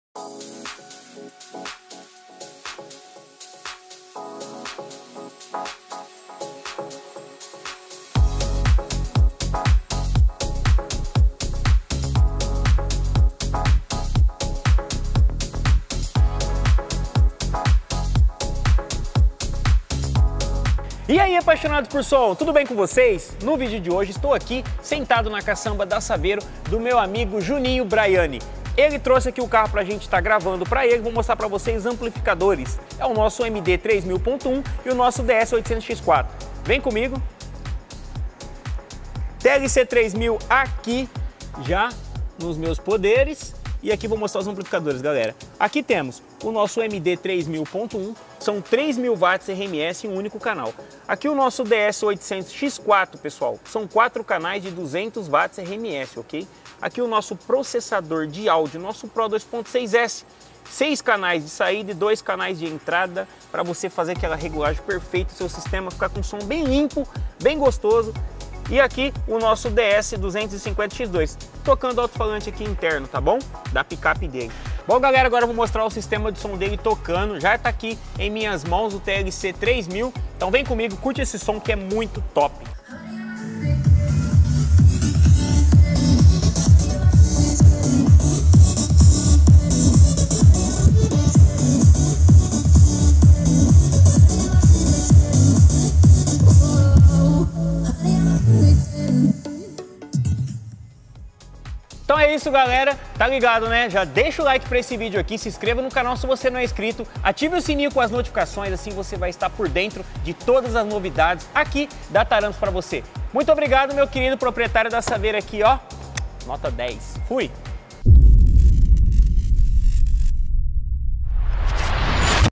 Saveiro com Módulos Taramps | Som do Juninho
Olá apaixonados por som!
Veja mais uma saveiro com Módulos Taramps!

Módulo amplificador Taramps 3000 watts MD 3000.1
Módulo amplificador Taramps 800 watts DS 800x4
Módulo amplificador Taramps 250 watts DS 250x2
e processador de áudio PRO 2.6S